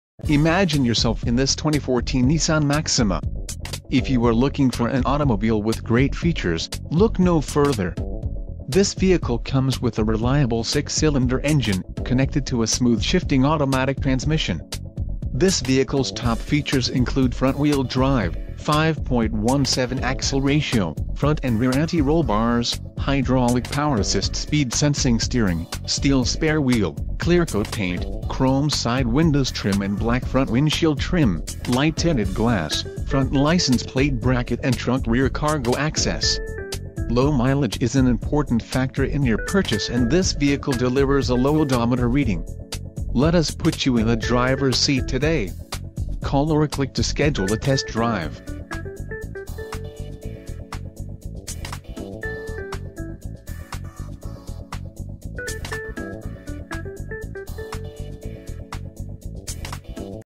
2014 Nissan Maxima 3.5 SV - Kelly Car - Emmaus, PA 18049
2014 Nissan Maxima 3.5 SV Condition: New Mileage: 15 Transmission: CVT Exterior Color: Crimson Interior Color: Charcoal Doors: 4 Stock: N44091 Cylinders: 6